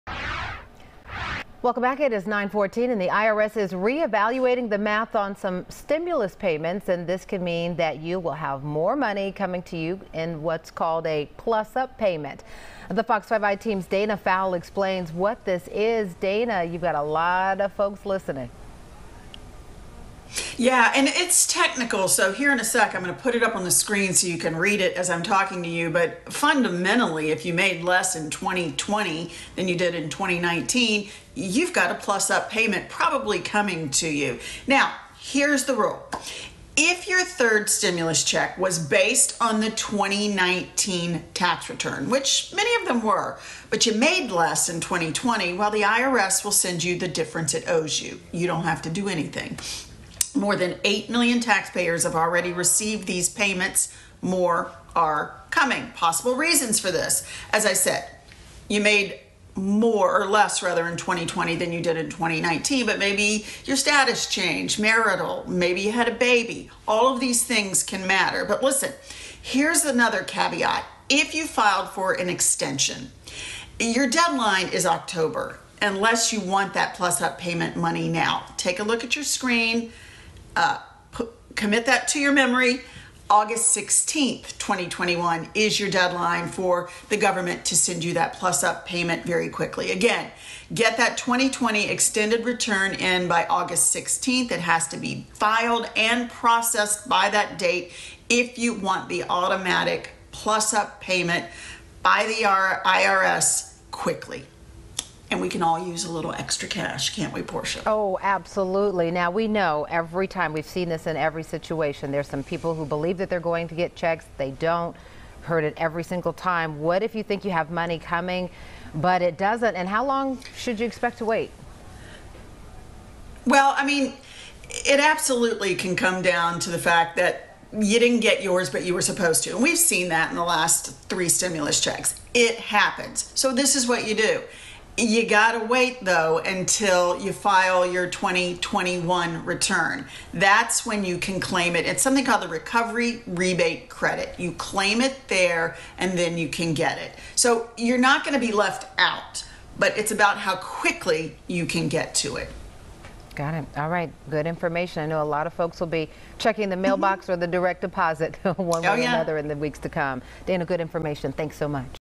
I-Team: IRS may be sending Georgians Plus Up payments
by Dana Fowle

The IRS is reevaluating the math on some stimulus payments and this can mean you have more money coming to you. More than 8 million taxpayers have already received these payments and more are coming.